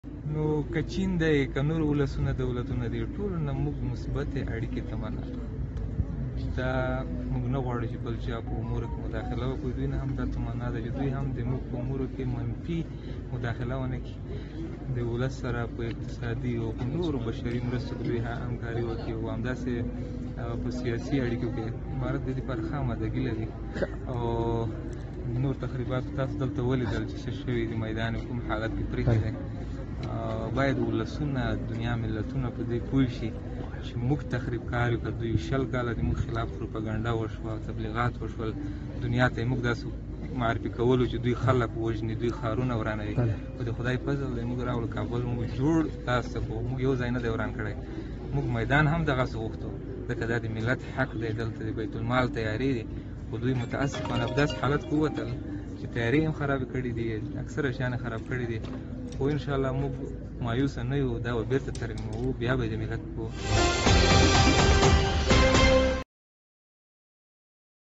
"Талибан": мы хотим иметь прекрасные отношения с Китаем
Афганистан хочет иметь прекрасные отношения с Китаем, об этом Анас Хаккани, высокопоставленный представитель движения "Талибан", сообщил сегодня корреспондентам CGTN в аэропорту Кабула. Подробности в нашем видео.